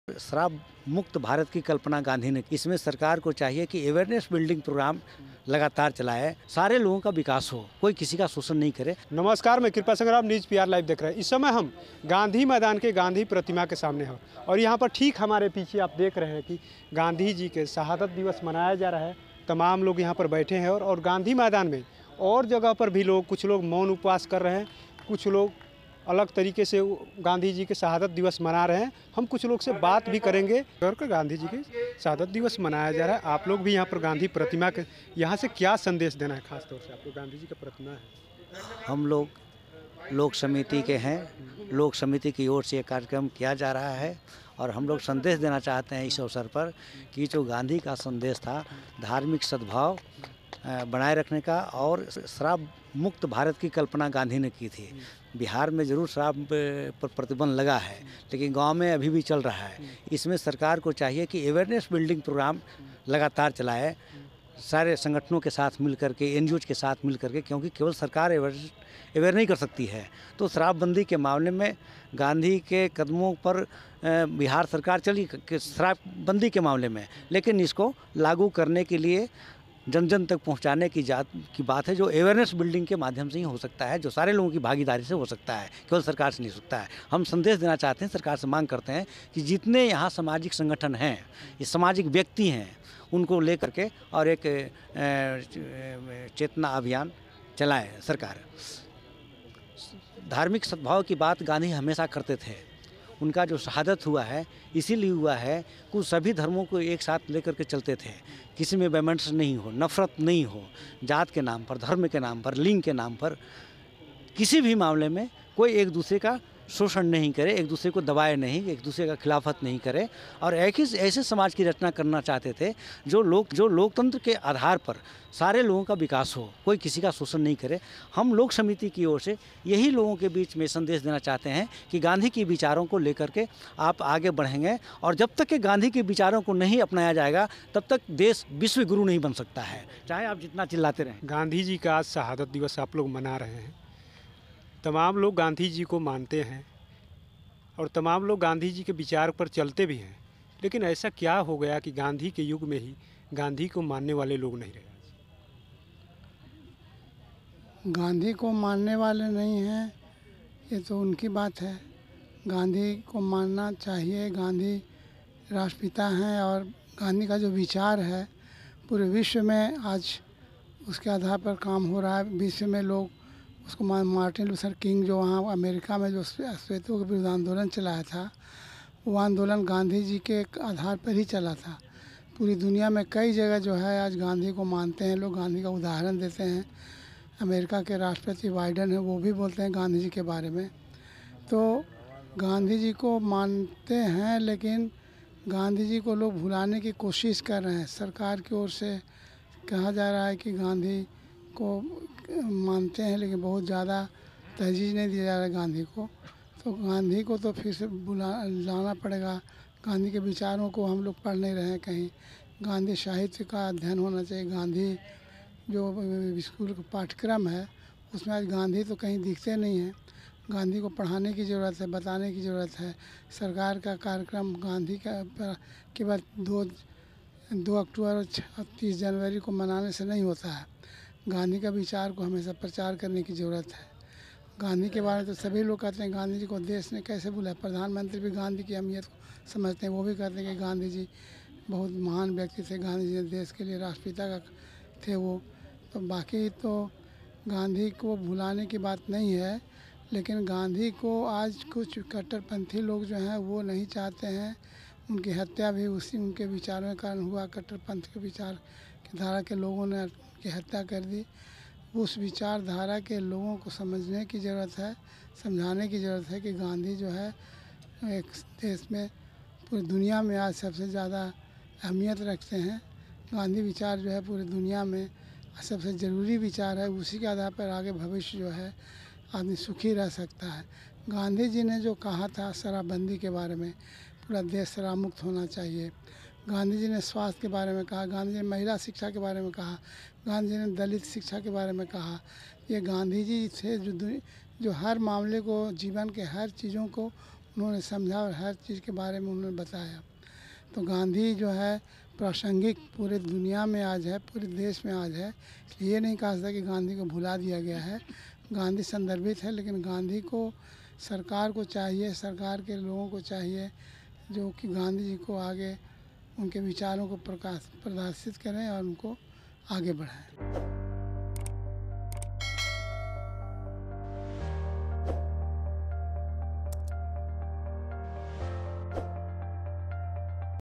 Gandhi Maidan में मनाई गयी Mahatma Gandhi की शहादत दिवस ! कर रहे विशेष मांग...
धन्यवाद
टीम NewsPR